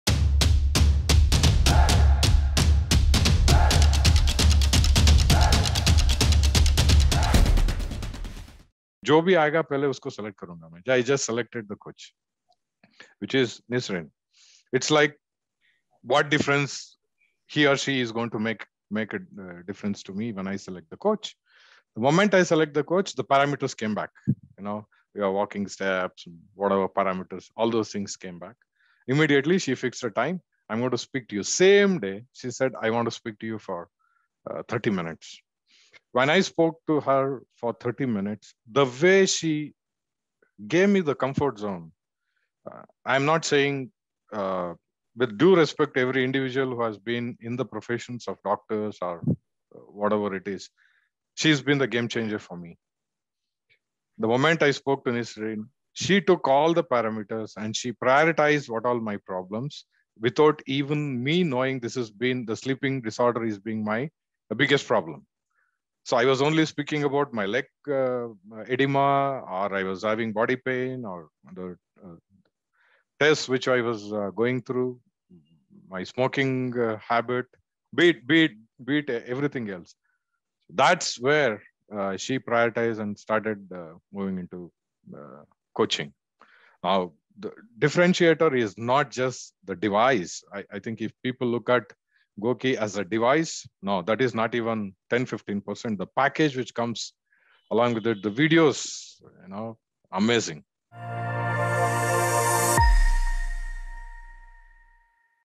How Is The GOQii Tracker Different From Other Devices?
Our GOQii Player Rudresh Basavarajappa talks about why he prefers the GOQii tracker above other trackers in the market.

To watch the entire conversation between Rudresh Basavarajappa and our Founder & CEO Vishal Gondal,

Make a positive lifestyle change by downloading the GOQii App